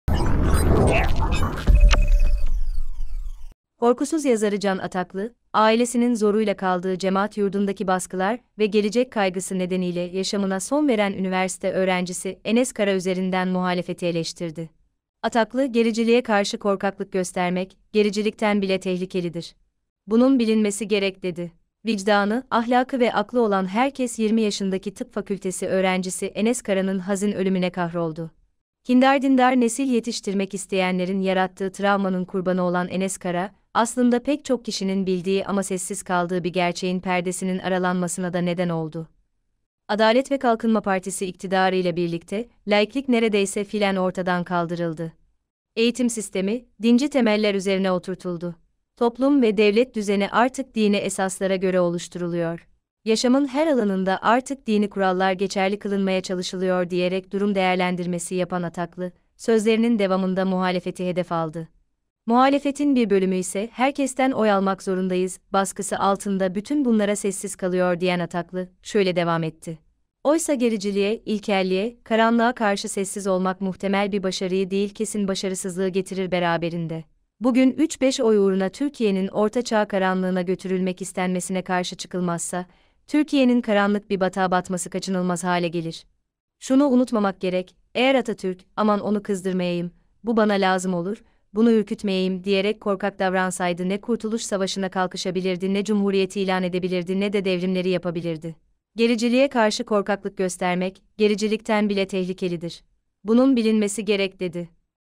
Can Ataklı'dan "korkaksınız" tepkisi
Korkusuz yazarı Can Ataklı, Enes Kara üzerinden muhalefeti hedef alarak “Gericiliğe karşı korkaklık göstermek, gericilikten bile tehlikelidir. Bunun bilinmesi gerek” dedi.